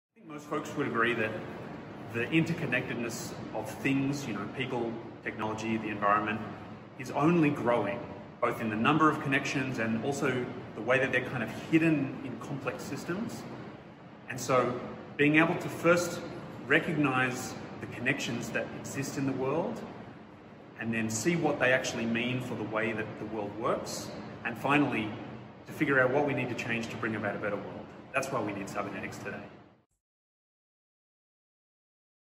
Why do you think cybernetics is important for the 21st century? Speaker: Ben Swift
Listen to Ben talk about why cybernetics is important for the 21st century. Share your thoughts on why cybernetics is important with us!